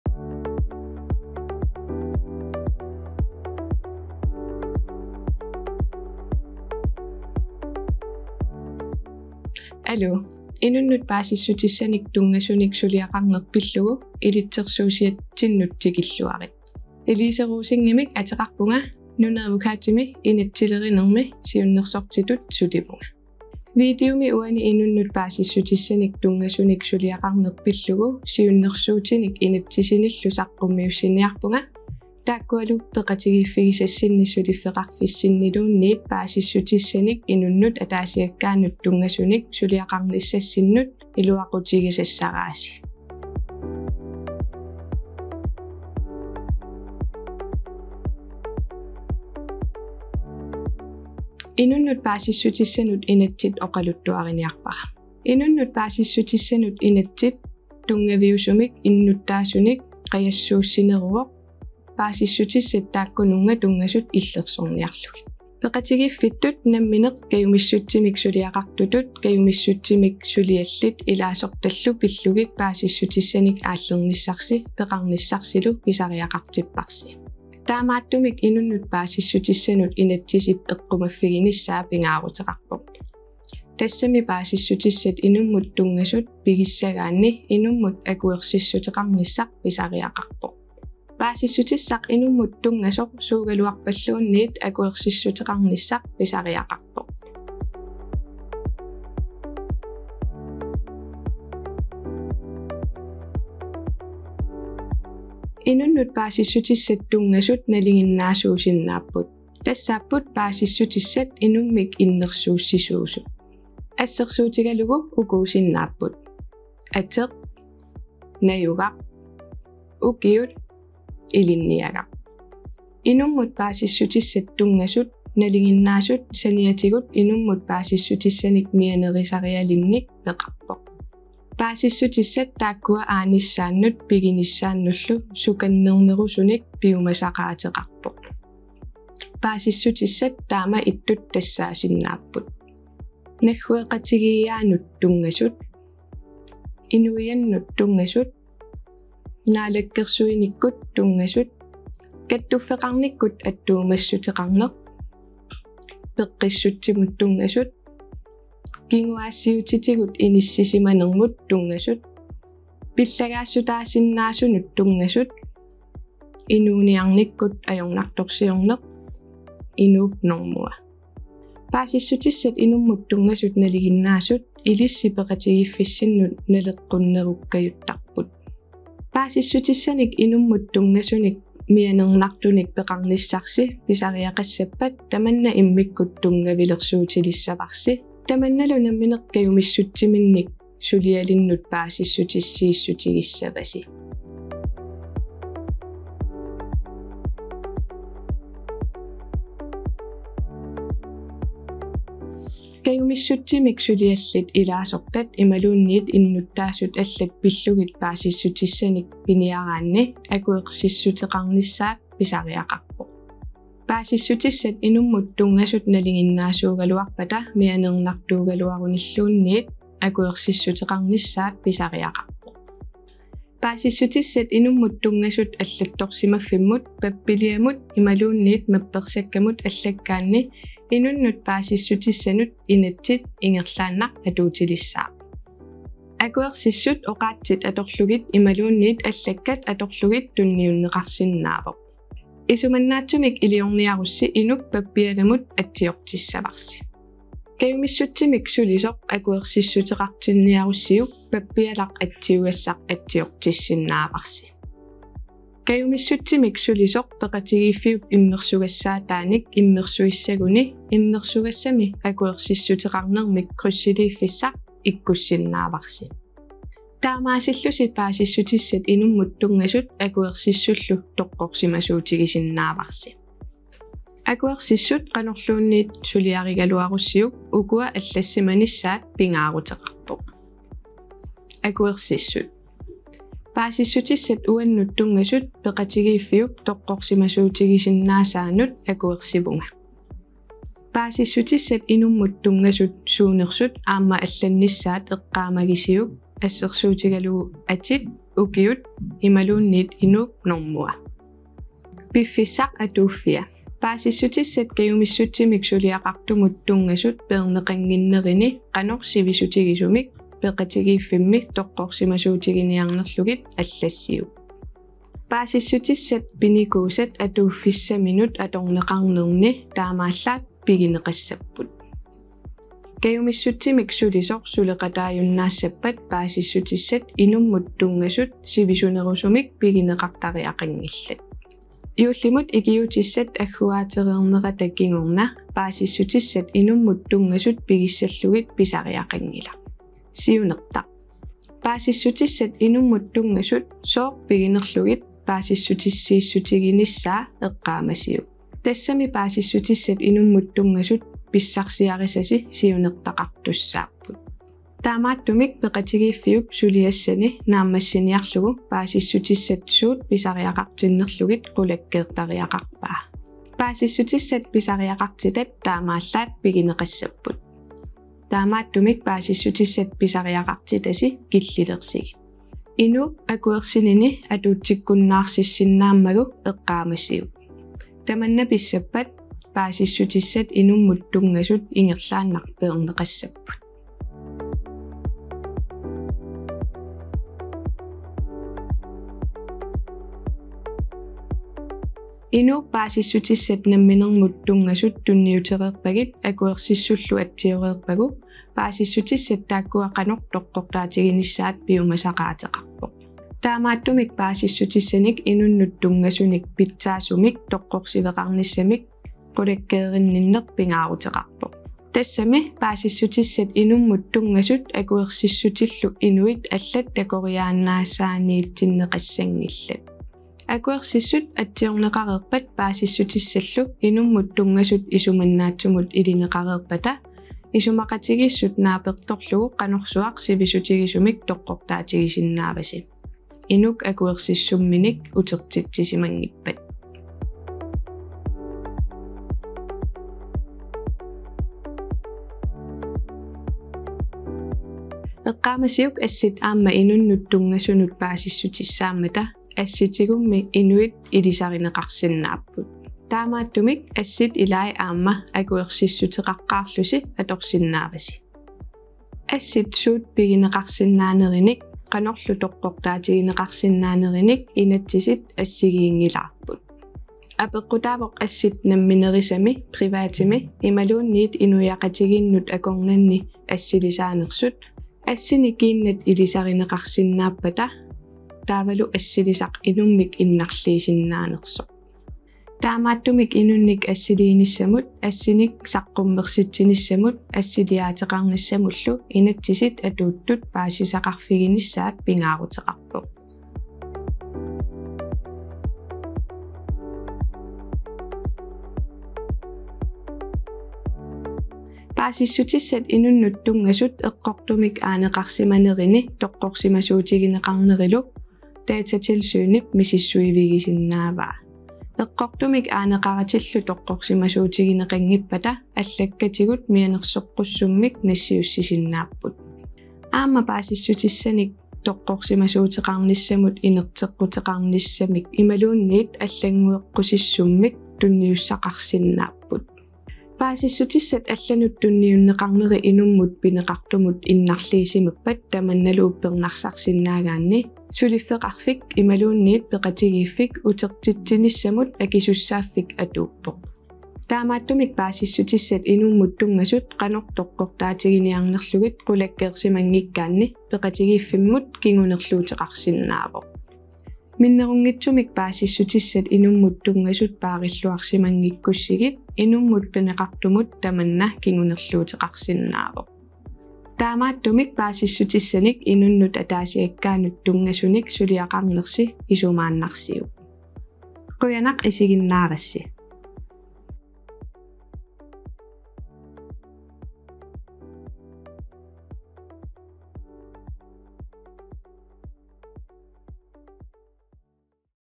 Inunnut paasissutissanik tunngasunik suliaqarneq - Håndtering af Personoplysninger og Persondata
Hvordan er reglerne når du som forening eller organisation skal håndtere oplysninger om frivillige og medlemmer.